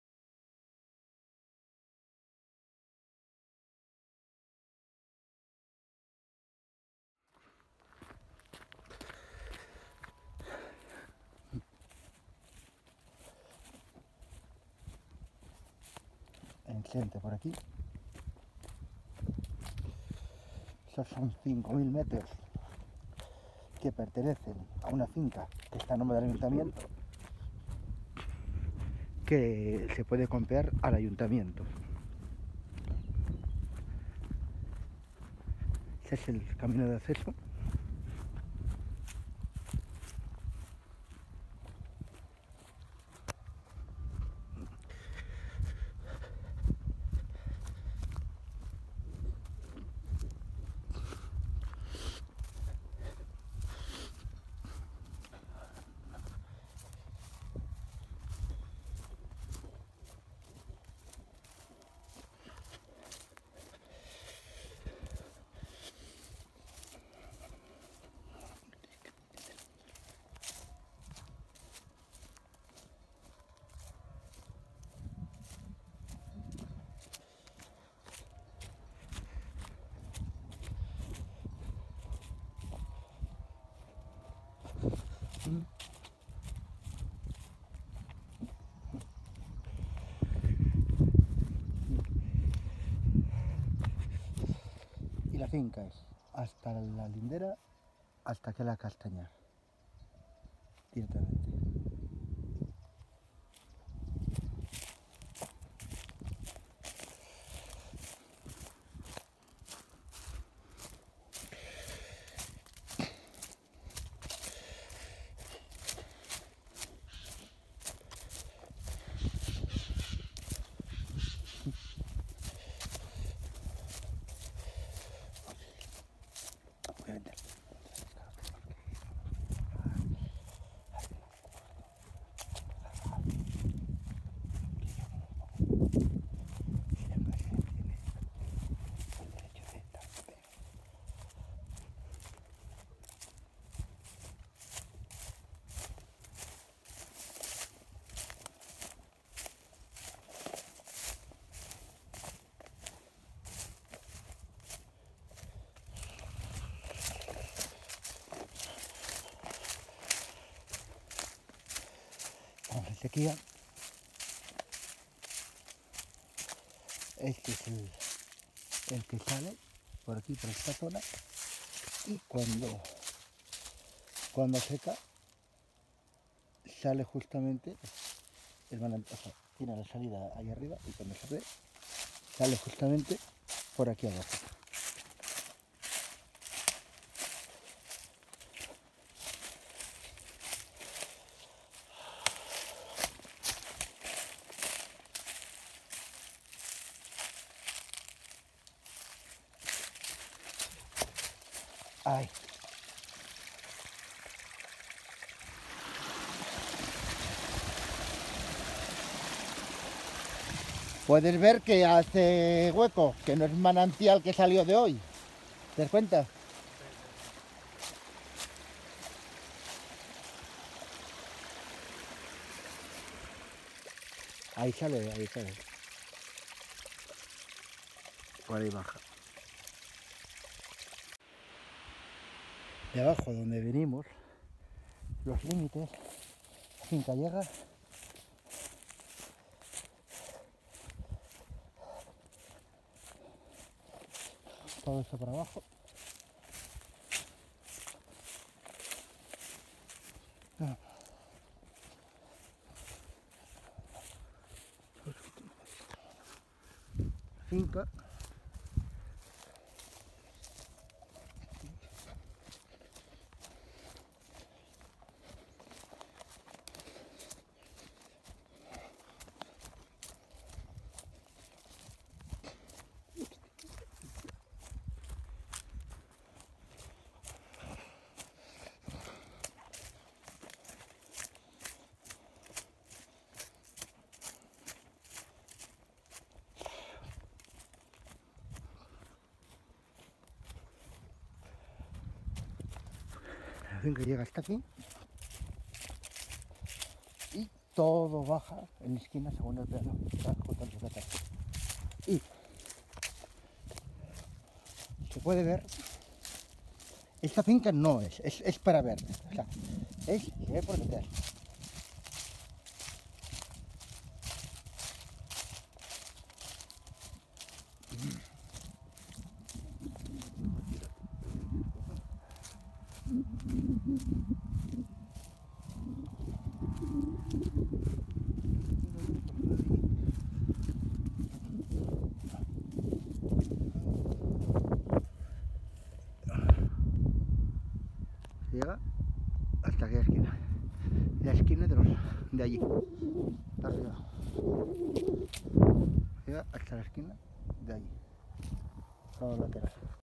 Secaes con manantial finca agraria
Finca Agraria con Manantial, se vende en Asturias, propiedad ideal para agro turismo en Asturias. Informa Antonio Con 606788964